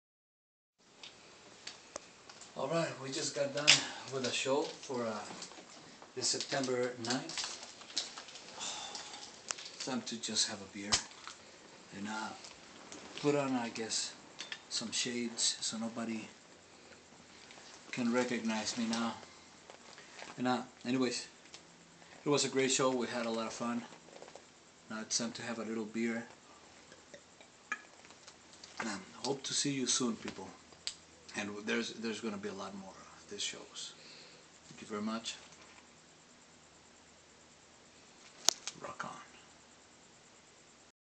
Gabe psyko circus 9/10/11 after show.
Getting home at 02:49 am after our awsome show, I was beat and thirsty.